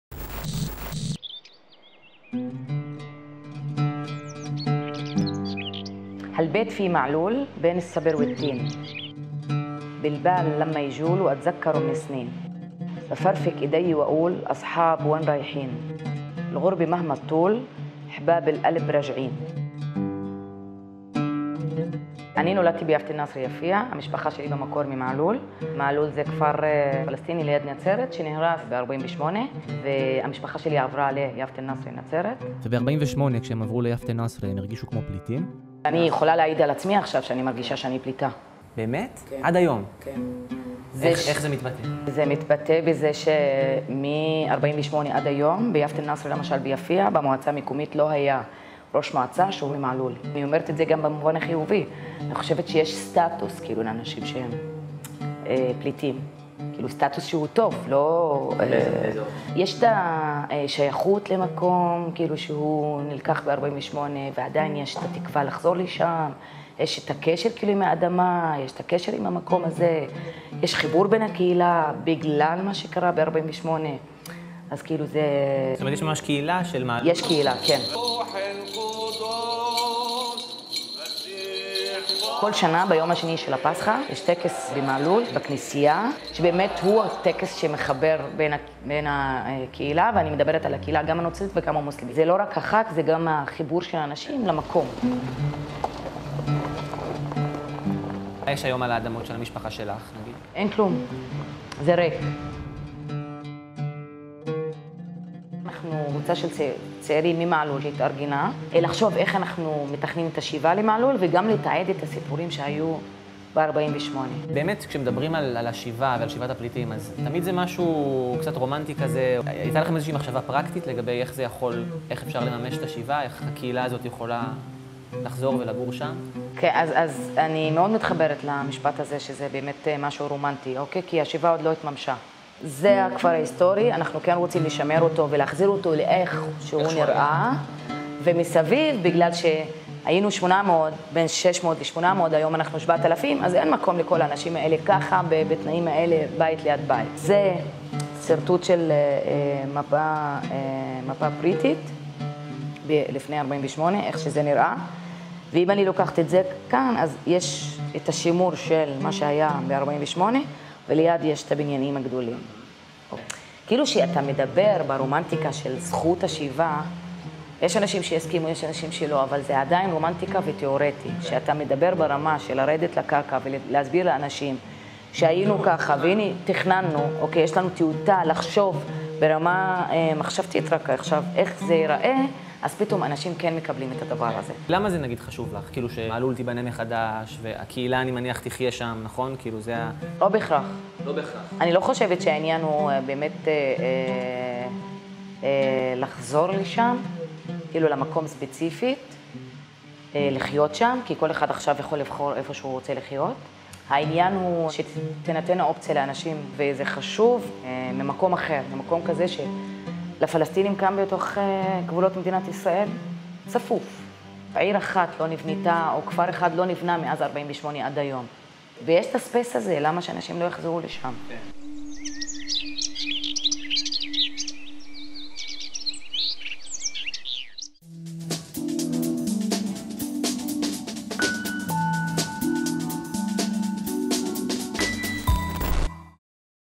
Talking about Return - Rawan Bisharat
Every year at Easter, a ceremony takes place in Ma'alul that connects the community of displaced persons and their descendants to the village from which they were expelled.  This is not just a romantic-symbolic act, the community also has a practical plan for realizing the return. A conversation with Rowan Bisharat, the daughter of an uprooted family from the village of Ma'lul.

יש לכם טלגרם? מוזמנים להירשם לערוץ ולהתעדכן באופן שוטף בכל הפעיליות של הטלוויזיה החברתית